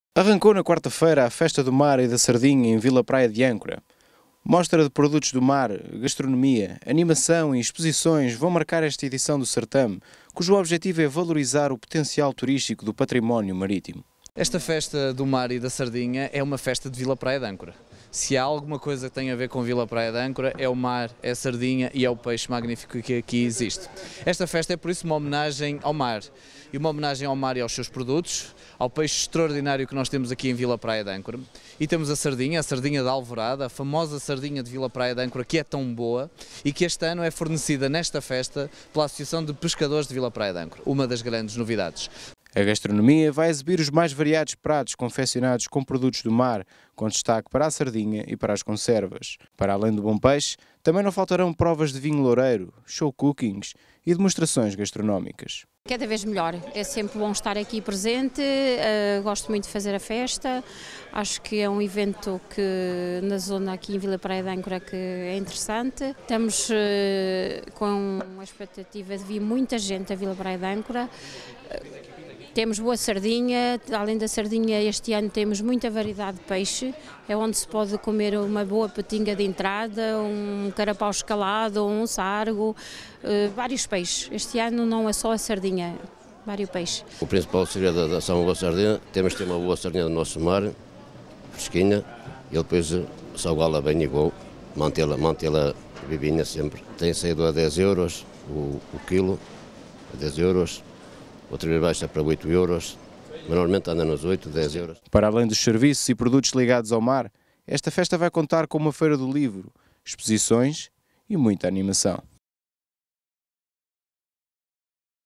Festa do Mar e da Sardinha anima Vila Praia de Âncora | Altominho TV
A Festa do Mar e da Sardinha vai animar Vila Praia de Âncora até ao dia 29 de Junho. Este certame aposta nas fortes ligações que o concelho mantém, desde sempre, com o mar.